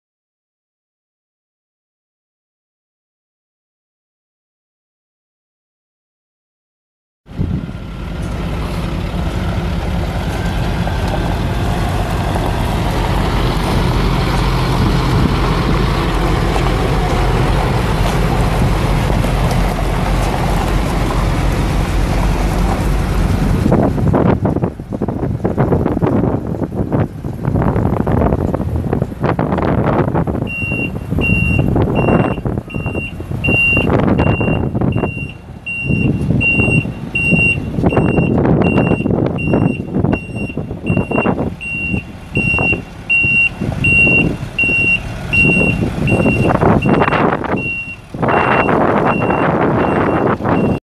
2002 Chevrolet C6500 service truck with crane for sale | no-reserve auction December 8, 2016
Duit Holdings, Inc Surplus Construction Equipment Auction

2002 Chevrolet C6500 service truck with crane
287,561 miles on odometer
Caterpillar 3126 7.2L six cylinder turbo diesel engine
330 HP
Serial CXM55169
Six speed manual transmission
Single axle
AC and heat
Auxiliary controls
Front tow hooks
PTO
Case Co. B2T1086LB service bed
15'L
Slide-out drawers
Utility rack
Work lights
(2) mounted vises
Rawson Koenig top-mounted toolbox
Ingersoll Rand 2475E16 air compressor
Hydraulic fluid reservoir
Torch bottle rack
Auto Crane 8406H crane
27.5' boom length
8,400 lbs max lift capacity
Remote control
(2) outriggers
Serial 8406H-0403-047
Dual fuel tanks
Receiver hitch
Spring suspension
Hydraulic brakes
182" wheelbase
10R22.5 tires
GVWR 25,950 lbs
Unit# 775.134
Oklahoma title